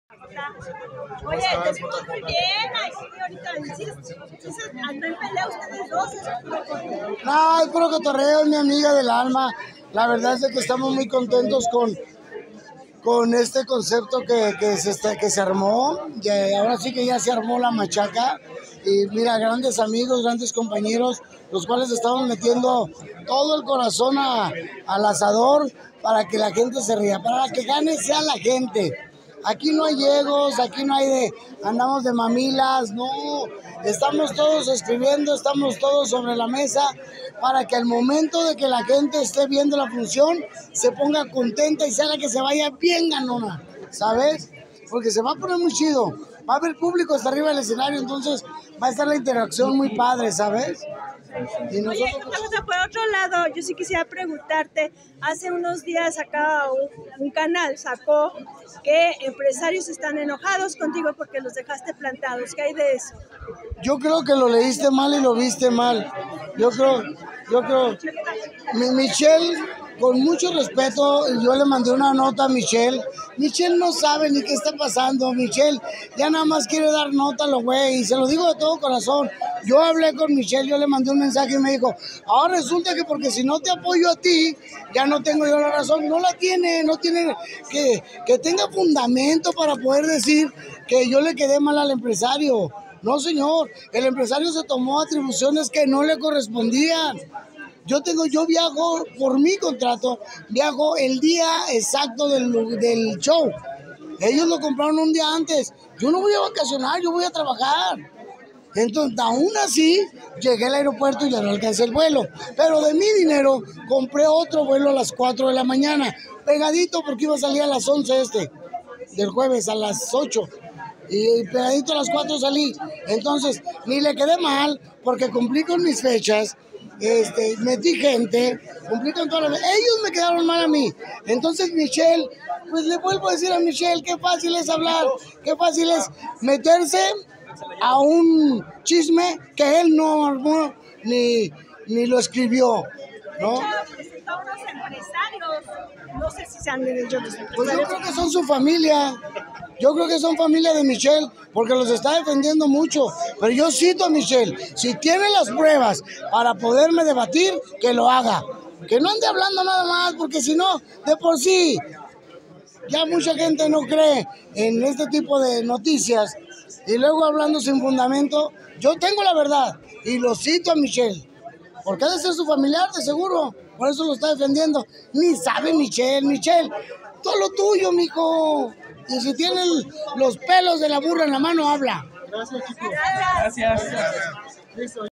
El JJ reta a Michelle Rubalcava a decir la verdad! Ya ni te creen, le dice! Ni sabes!, remata!
El JJ reta a Michelle Rubalcava a que diga la verdad, pues, es mentira que dejó plantado a empresarios